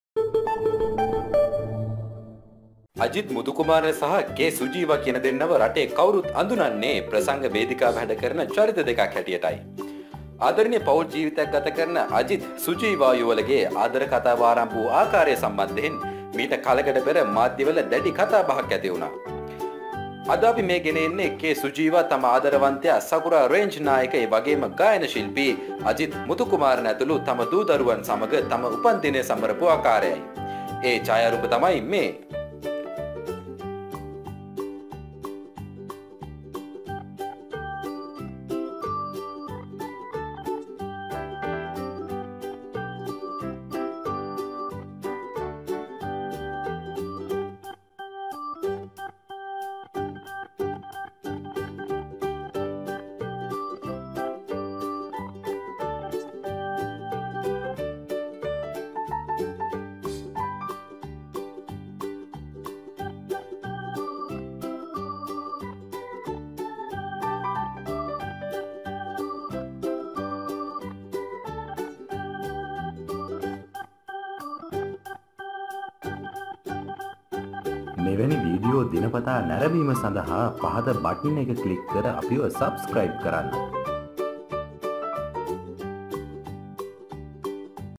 අජිත් මුතුකුමාරනගේ බිරිඳ K. සුජීවා අද දින සිය දියණියන් දෙදෙනා සමඟ උපන්දිනය සැමරූ හැටි
අජිත් මුතුකුමාරනගේ බිරිඳ K. සුජීවා අද දින සිය දියණියන් දෙදෙනා සමඟ උපන්දිනය සැමරූ හැටි k සුජීවා තම ආදරවන්තයා සකුරා රේන්ජ් නායක , ගායන ශිල්පී අජිත් මුතුකුමාරණ ඇතුළු තම දු දරුවන් සමග තම උපන්දිනය සමරපු ආකාරයයි.ජායාරූප පහතින් නරඹන්න